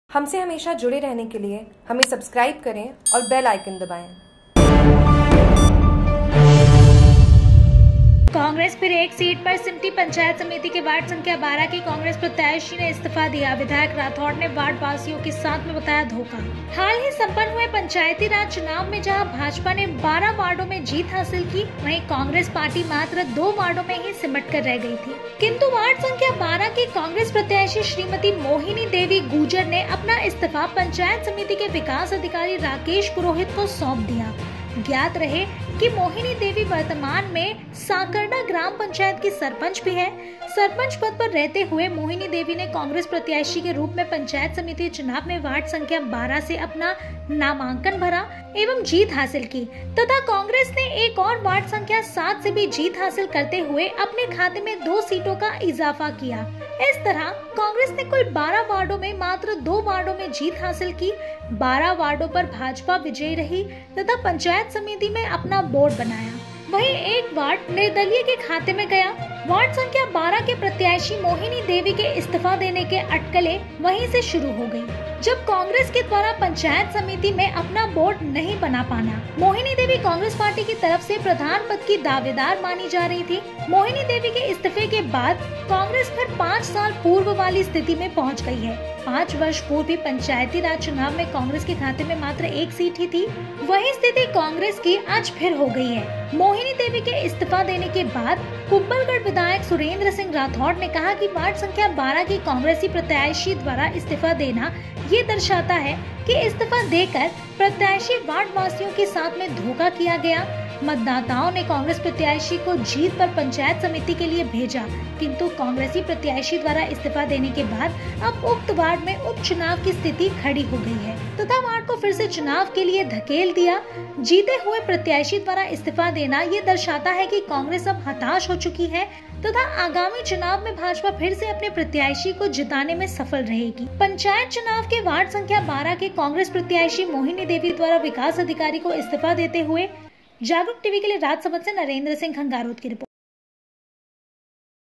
Jagruk TV : कांग्रेस फिर 1 सीट पर सिमटी,पंचायत समिति के वार्ड संख्या 12 की प्रत्याशी ने इस्तीफा दिया
राजसमंद /आमेट  हाल ही संपन्न हुए पंचायती राज चुनाव में जहां भाजपा ने 12 वार्डों में जीत हासिल की। वहीं कांग्रेस पार्टी मात्र 2 वार्डो में ही सिमट कर रह गई थी। किंतु वार्ड संख्या 12 की कांग्रेस प्रत्याशी श्रीमती मोहिनी देवी गुर्जर ने अपना इस्तीफ पंचायत समिति के विकास अधिकारी राकेश पुरोहित को सौंप दिया। ज्ञात रहे कि मोहिनी देवी वर्तमान में साकरड़ा ग्राम पंचायत की सरपंच भी है। सरपंच पद पर रहते हुए मोहिनी देवी ने कांग्रेस प्रत्याशी के रूप में पंचायत समिति चुनाव में वार्ड संख्या 12 से अपना नामांकन भरा एवं जीत हासिल की तथा कांग्रेस ने एक और वार्ड संख्या 7 से भी जीत हासिल करते हुए अपने खाते में 2 सीटों का इजाफा किया ।इस तरह कांग्रेस ने कुल 15 वार्डों में मात्र 2 वार्डो में जीत हासिल की।12 वार्डो पर भाजपा विजय रही तथा पंचायत समिति में अपना बोर्ड बनाया। वही एक वार्ड निर्दलीय के खाते में गया। वार्ड संख्या 12 के प्रत्याशी मोहिनी देवी के इस्तीफे देने की अटकलें वही से शुरू हो गई जब कांग्रेस के द्वारा पंचायत समिति में अपना बोर्ड नहीं बना पाना।मोहिनी देवी कांग्रेस पार्टी की तरफ से प्रधान पद की दावेदार मानी जा रही थी।मोहिनी देवी के इस्तीफे के बाद कांग्रेस फिर 5 साल पूर्व वाली  स्थिति में  पहुंच गई है। 5 वर्ष पूर्व भी पंचायती राज चुनाव में कांग्रेस के खाते में मात्र 1 सीट ही थी। वही स्थिति कांग्रेस की आज फिर हो गई है।मोहिनी देवी के इस्तीफा देने के बाद कुंभलगढ़ विधायक सुरेंद्र सिंह राठौड़ ने कहां की वार्ड संख्या 12 की कांग्रेसी प्रत्याशी द्वारा इस्तीफा देना यह दर्शाता है कि   इस्तीफा देकर प्रत्याशी वार्ड वासियों के साथ में धोखा किया गया। मतदाताओं ने कांग्रेस प्रत्याशी को जीता कर पंचायत समिति के लिए भेजा।किंतु कांग्रेसी प्रत्याशी द्वारा इस्तीफ़ा देने के बाद अब उक्त वार्ड में उपचुनाव की स्थिति खड़ी हो गई है।तथा वार्ड को फिर से चुनाव के लिए धकेल दिया।जीते हुए प्रत्याशी द्वारा इस्तीफा देना यह दर्शाता है कि कांग्रेस अब हताश हो चुकी है तथा आगामी उपचुनाव में भाजपा फिर से अपने प्रत्याशी को जिताने में सफल रहेगी।

 फोटो केप्सन

 पंचायत समिति के वार्ड संख्या 12 के कांग्रेस प्रत्याशी मोहनी देवी द्वारा विकास अधिकारी को इस्तीफा देते हुए। नरेंद्र सिंह खंगारोत जागरूक टीवी की रिपोर्ट राजसमंद से